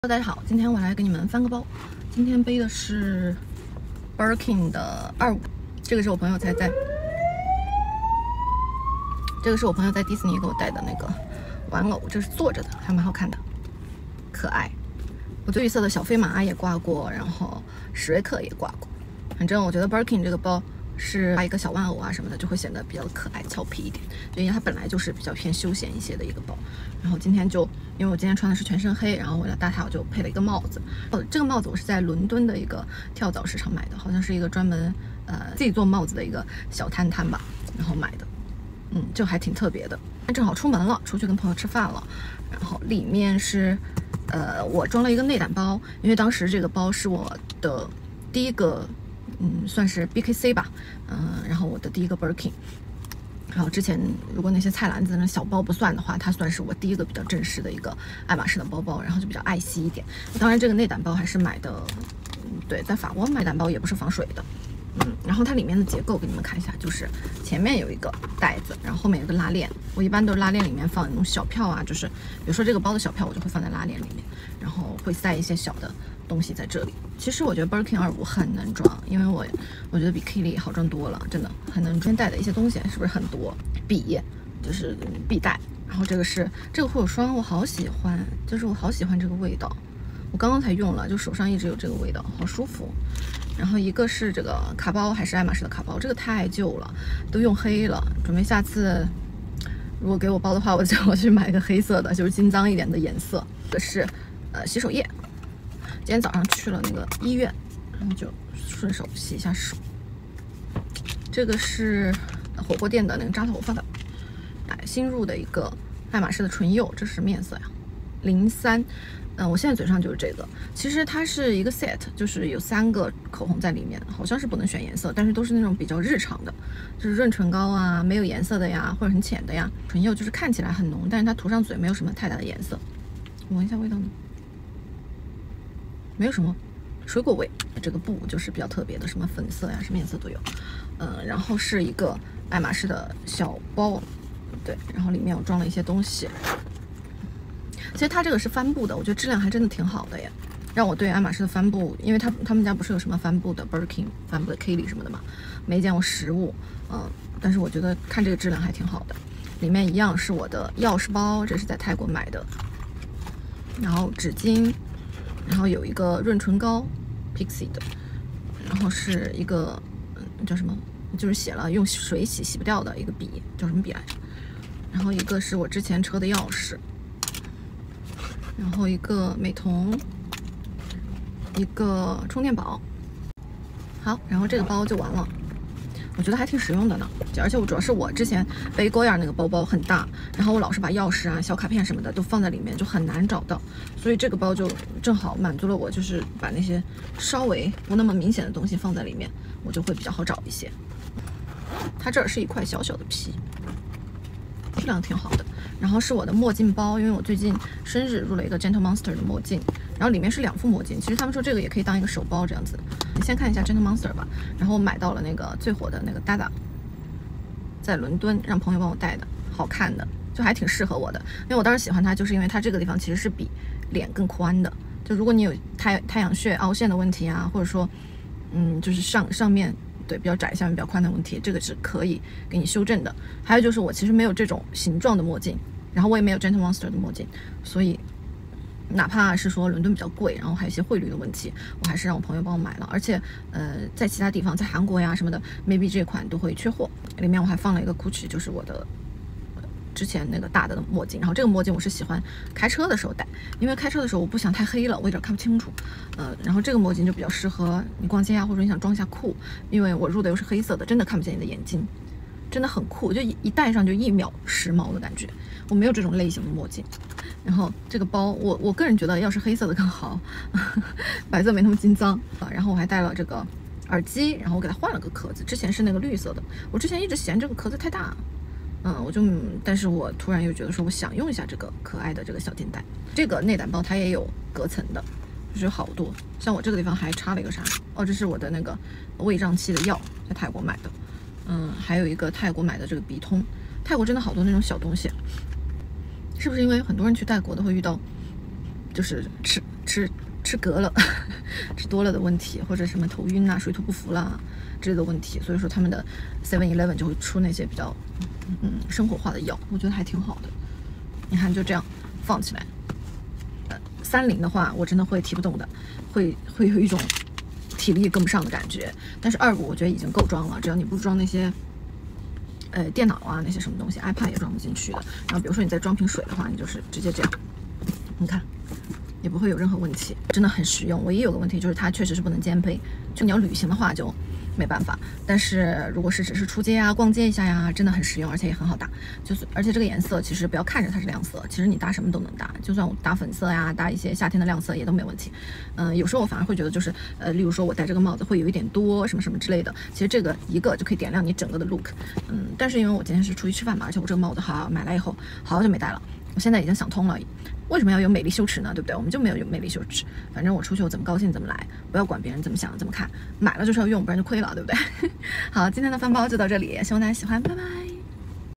日常翻包｜ What’s in my bag | birkin 25 孔雀绿｜ Gentle monster Dada ｜ 随身好物｜爱马仕唇釉
护手霜 blanche
墨镜dada
爱马仕 唇釉03
上衣 cos